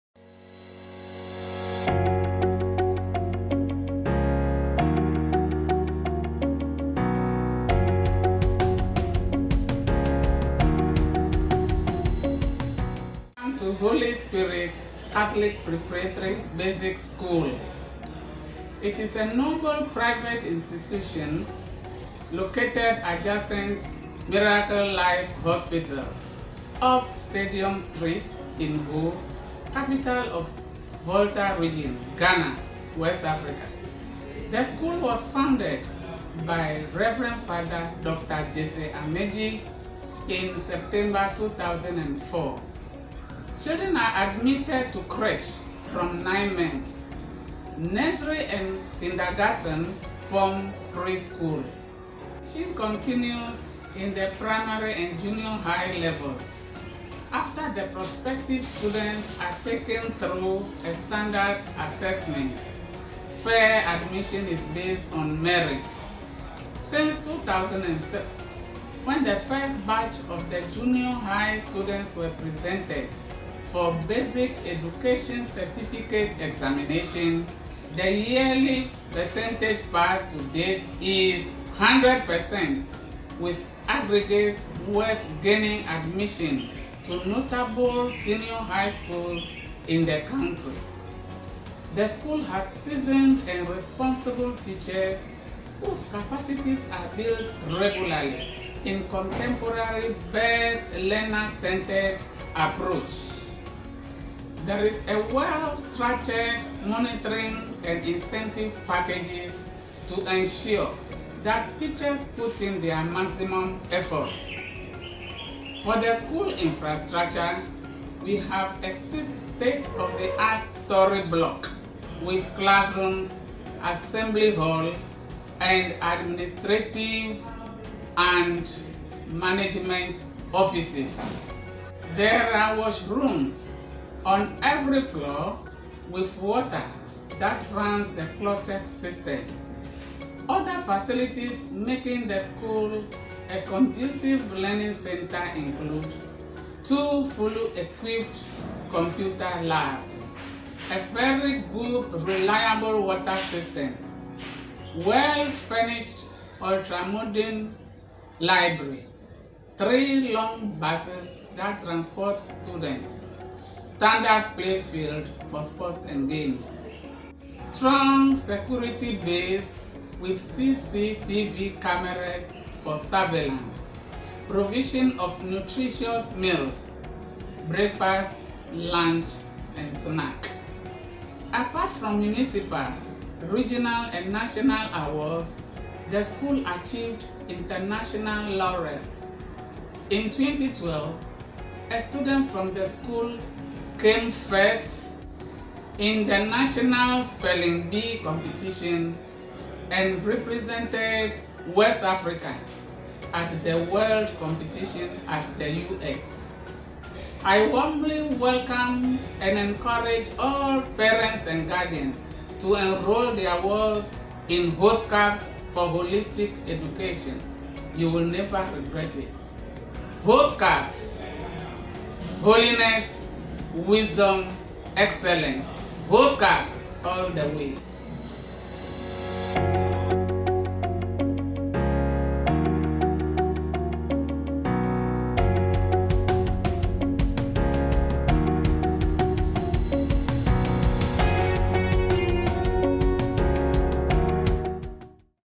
holy spirit catholic preparatory school ho
Interview with the Holy Spirit Catholic Preparatory School's Administrator. The school is located in the heart of the Volta Regional Capital, Ho in Ghana, West Africa.